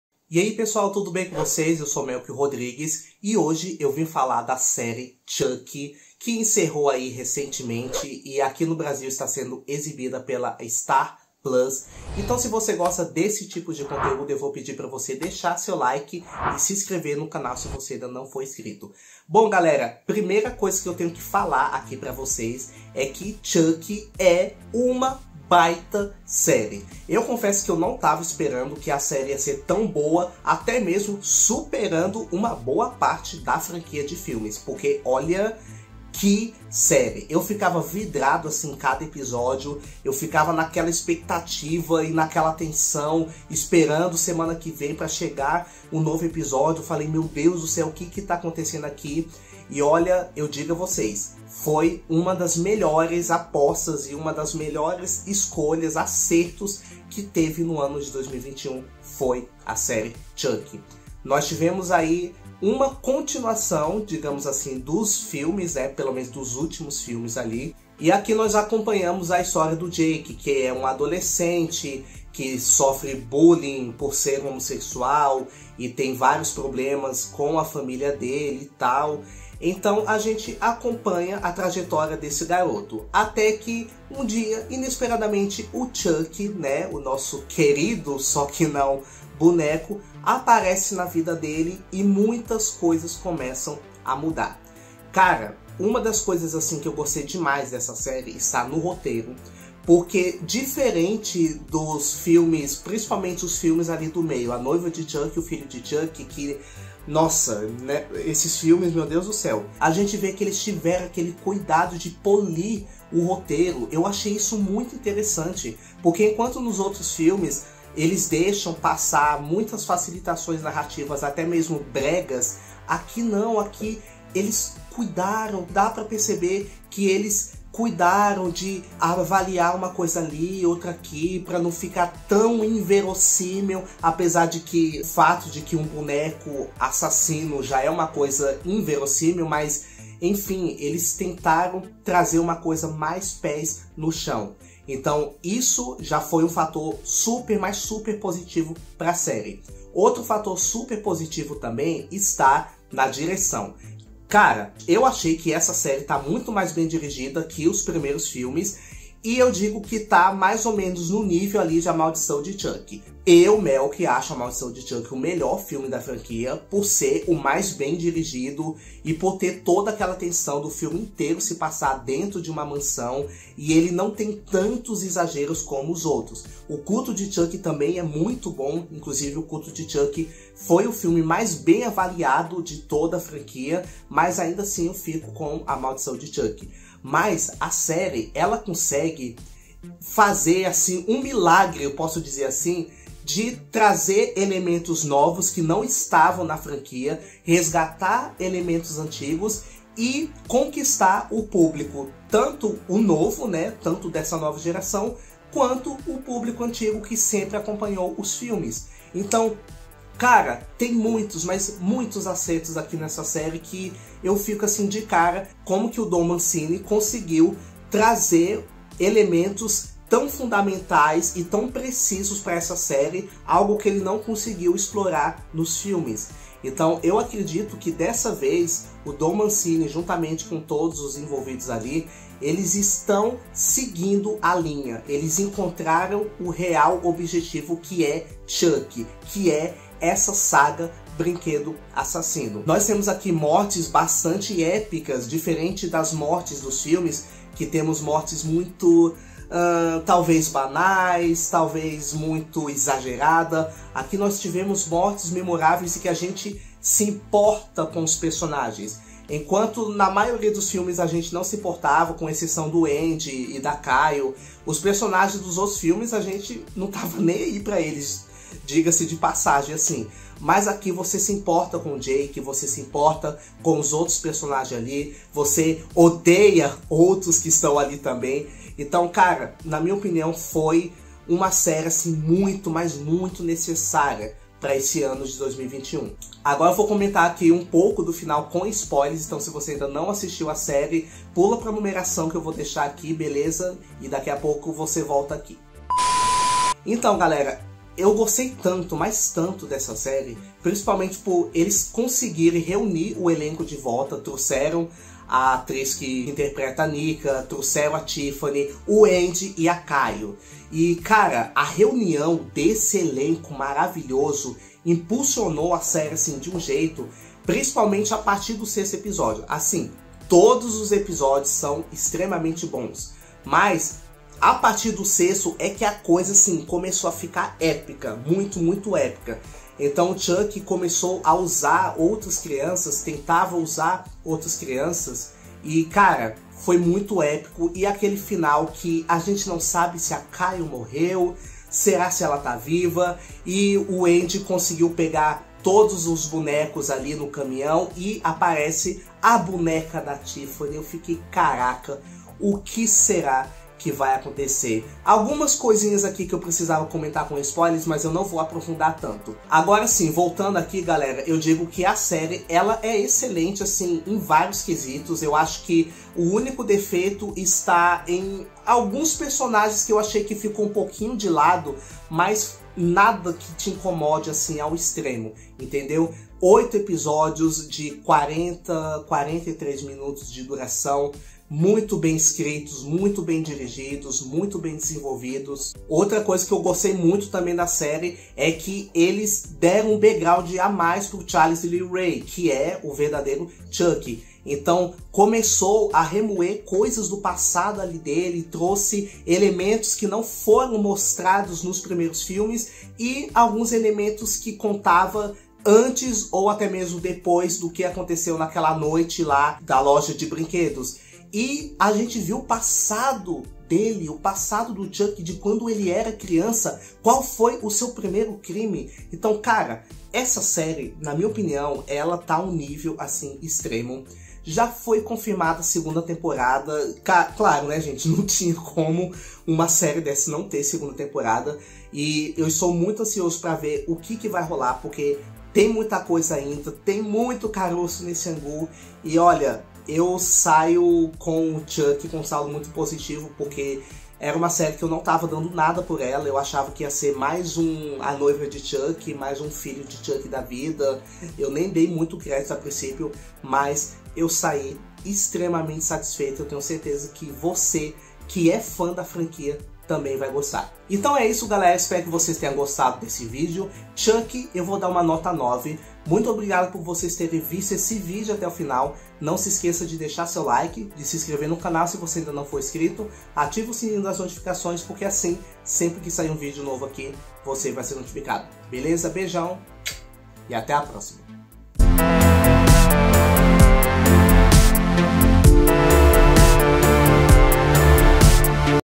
CHUCKY É UMA DAS MELHORES SÉRIES DE 2021 E EU POSSO PROVAR! | CRÍTICA COM E SEM SPOILERS
Fala, pessoal! Hoje eu vim trazer pra vocês a crítica da primeira temporada da série "Chucky", sequência da série clássica dos filmes "Brinquedo Assassino".

► Ganhe 30 dias de assinatura grátis da Prime Video